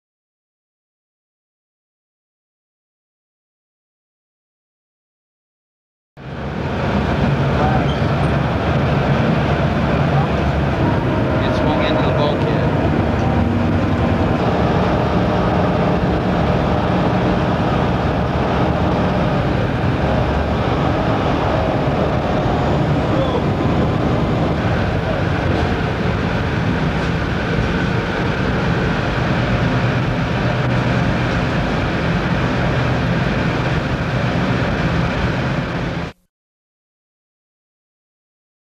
5 Seasons Book video clip 20 - Ryerson bugs
S/S Edward L  Ryerson upbound Soo Locks with bugs on deck 1993 - Five Seasons on Steel Decks book companion video.